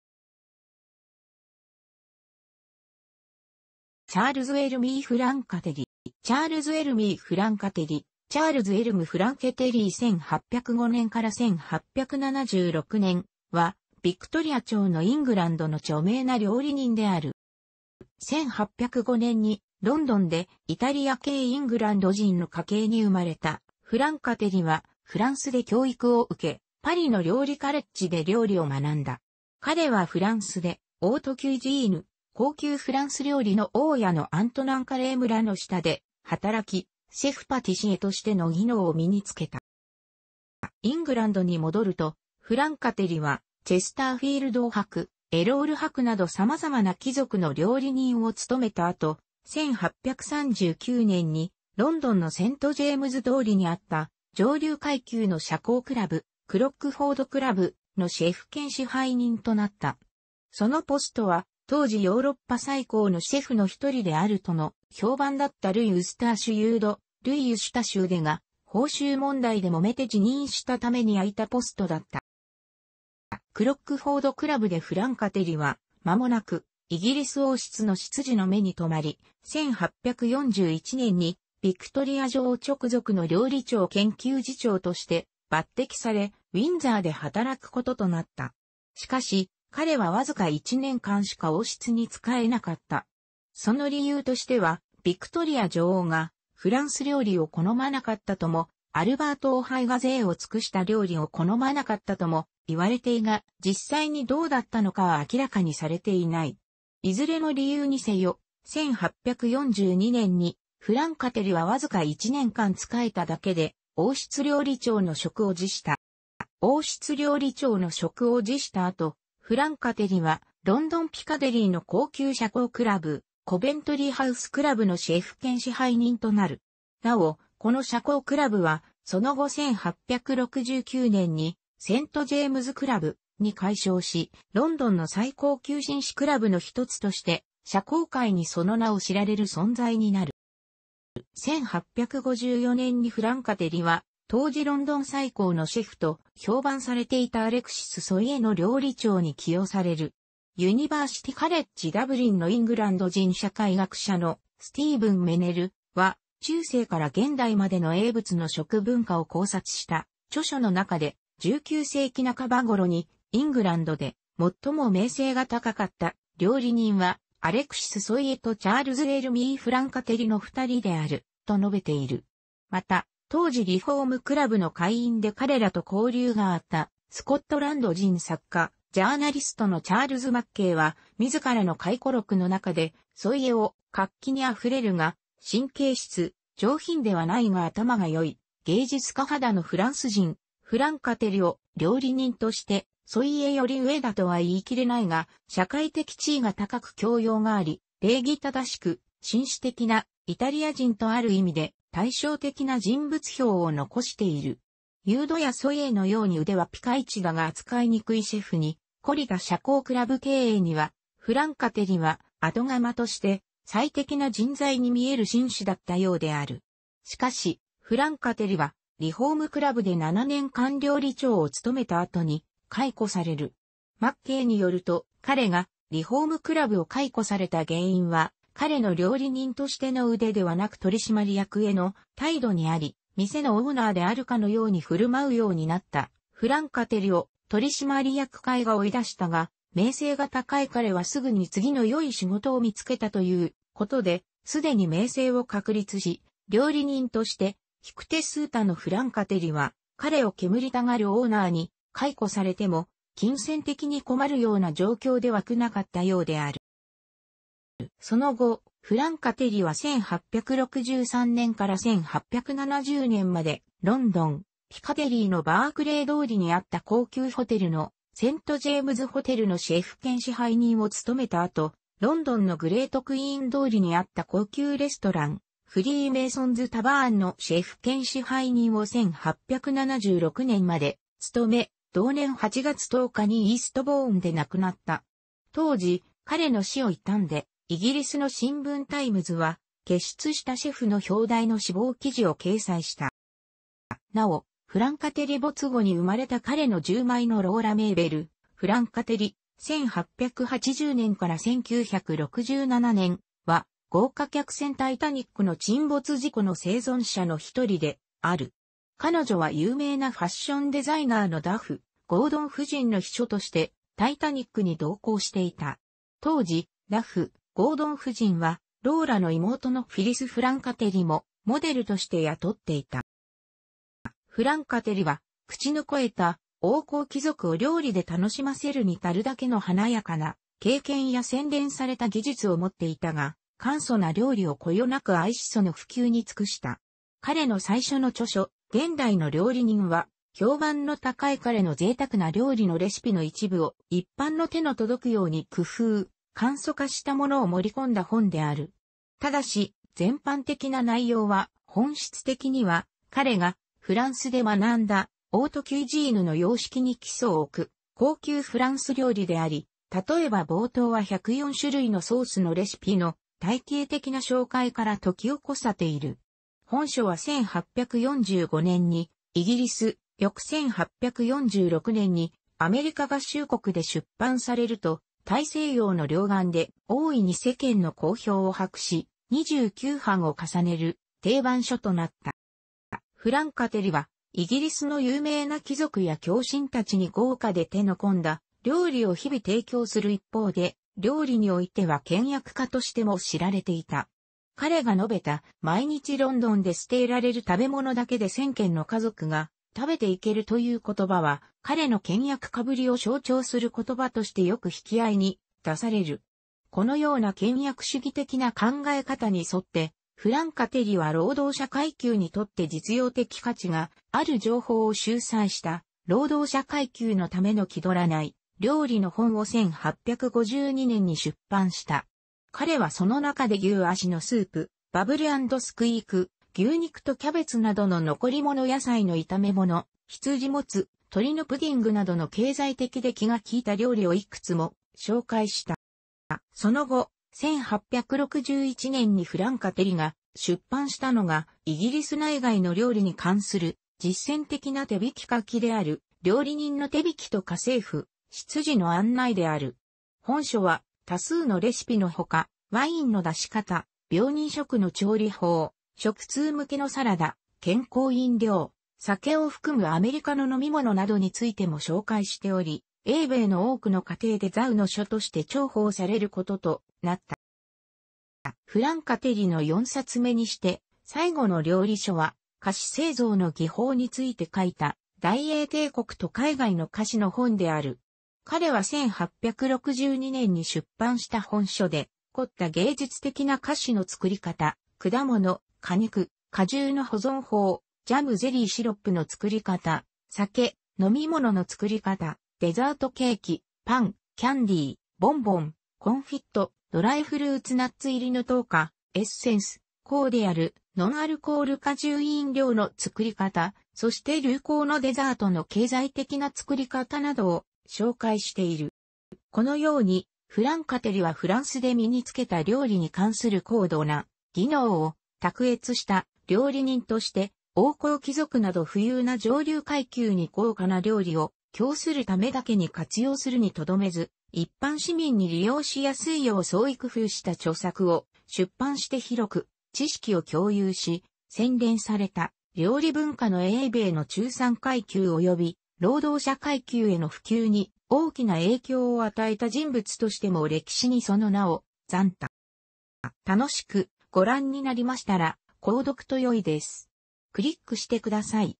チャールズ・エルミー・フランカテリ
チャールズ・エルミー・フランカテリ（Charles Elmé Francatelli 1805年 - 1876年）は 、ヴィクトリア朝のイングランドの著名な料理人である。

1805年にロンドンでイタリア系イングランド人の家系に生まれたフランカテリは、フランスで教育を受け、パリの料理カレッジで料理を学んだ。彼はフランスでオートキュイジーヌ（高級フランス料理）の大家のアントナン・カレーム (Marie-Antoine Carême) らの下で働きシェフ・パティシエとしての技能を身につけた。

イングランドに戻ると、フランカテリはチェスターフィールド伯()、ダドリー伯 ()、エロール伯()、パースシャーのキナード卿 () など様々な貴族の料理人を務めた後、1839年にロンドンのセントジェームズ通りにあった上流階級の社交クラブ「クロックフォード・クラブ」 () のシェフ兼支配人となった。そのポストは、当時ヨーロッパ最高のシェフの一人であるとの評判だったルイ・ウスターシュ・ユード（Louis Eustache Ude） が報酬問題でもめて辞任したために空いたポストだった。

クロックフォード・クラブでフランカテリは間もなくイギリス王室の執事の目にとまり、1841年にヴィクトリア女王直属の料理長兼給仕長として抜擢されウィンザーで働くこととなった。しかし、彼はわずか1年間しか王室に仕えなかった。その理由としては、ヴィクトリア女王がフランス料理を好まなかったとも、アルバート王配が贅を尽くした料理を好まなかったともいわれているが、実際にどうだったのかは明らかにされていない。いずれの理由にせよ、1842年にフランカテリはわずか1年間仕えただけで王室料理長の職を辞した。

王室料理長の職を辞した後、フランカテリはロンドン・ピカデリーの高級社交クラブ「コベントリーハウス・クラブ」 (Coventry House Club) のシェフ兼支配人となる。なお、この社交クラブはその後1869年に「セント・ジェームズ・クラブ」 () に改称し、ロンドンの最高級紳士クラブの一つとして社交界にその名を知られる存在になる。

1854年にフランカテリは当時ロンドン最高のシェフと評判されていたアレクシス・ソィエ () が辞任した後任としてロンドンのペルメルにある高級社交クラブ「リフォーム・クラブ」 () の料理長に起用される。ユニバーシティ・カレッジ・ダブリンのイングランド人社会学者のスティーヴン・メネル () は、中世から現代までの英仏の食文化を考察した著書の中で、「19世紀半ば頃にイングランドで最も名声が高かった料理人は、アレクシス・ソィエとチャールズ・エルミー・フランカテリの2人である」と述べている。また、当時リフォーム・クラブの会員で彼らと交流があったスコットランド人作家・ジャーナリストのチャールズ・マッケイは、自らの回顧録の中で、ソィエを「活気に溢れるが神経質、上品ではないが頭が良い、芸術家肌のフランス人」、フランカテリを「料理人としてソィエより上だとは言い切れないが、社会的地位が高く教養があり、礼儀正しく紳士的なイタリア人」とある意味で対照的な人物評を残している。ユードやソィエのように腕はピカイチだが使いにくいシェフに懲りた社交クラブ経営者には、フランカテリは後釜として最適な人材に見える紳士だったようである。
しかし、フランカテリはリフォーム・クラブで7年間料理長を務めた後に解雇される。マッケイによると、彼がリフォーム・クラブを解雇された原因は、彼の料理人としての腕ではなく取締役への態度にあり、「店のオーナーであるかのようにふるまうようになったフランカテリを取締役会が追い出したが、名声が高い彼は直ぐに次の良い仕事を見つけた」ということで、既に名声を確立し料理人として引く手数多のフランカテリは、彼を煙たがるオーナーに解雇されても金銭的に困るような状況では全くなかったようである。

その後フランカテリは1863年から1870年までロンドン、ピカデリーのバークレー通りにあった高級ホテルの「セントジェームズ・ホテル」 (St James's Hotel) のシェフ兼支配人を務めた後、ロンドンのグレートクイーン通りにあった高級レストラン「フリーメーソンズ・タヴァーン」 (Freemasons' Tavern) のシェフ兼支配人を1876年まで務め、同...